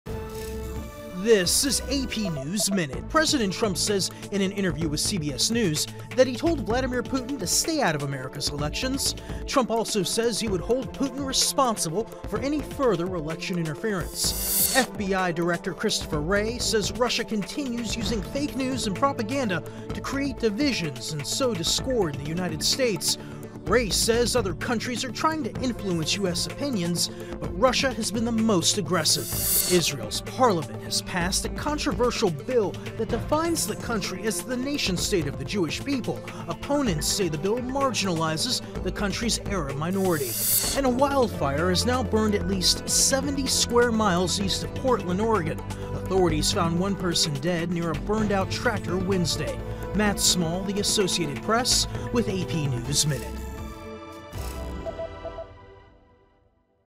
AP Top Stories July 19 A
(19 Jul 2018) Here's the latest for Thursday July 19th: Trump says he told Putin to stay out of US elections; FBI Director says Russia is still trying to sway opinions in US; Israeli parliament passes contentious Jewish nation bill; Massive wildfire in Oregon.